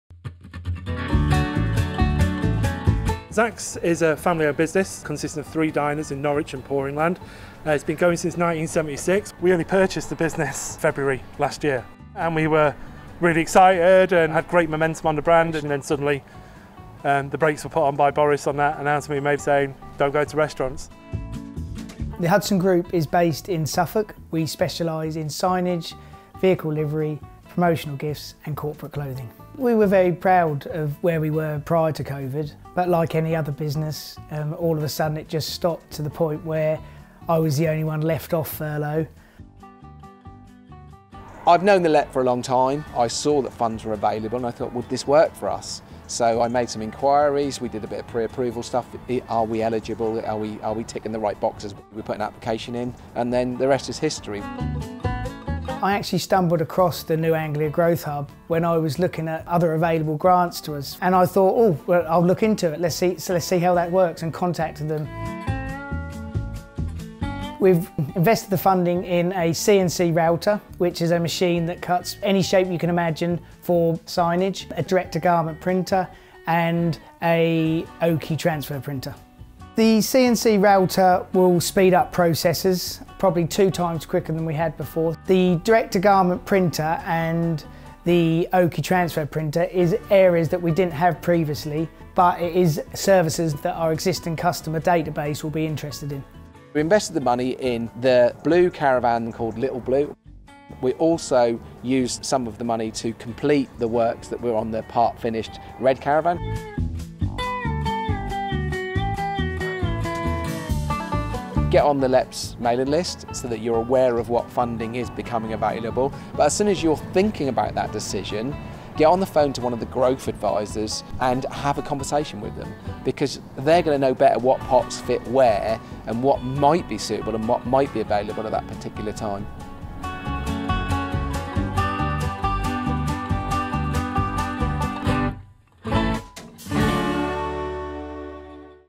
New Anglia LEP Recovery & Resilience Fund Zaks & Hudson version Sept 2021
We were commissioned to create a film that showed how three companies in the region had used funding to invest in their businesses so that they could survive and thrive during the COVID pandemic. It was fabulous to be able to visit Zaks in Poringland and the Hudson Group in Ipswich and Needham Market.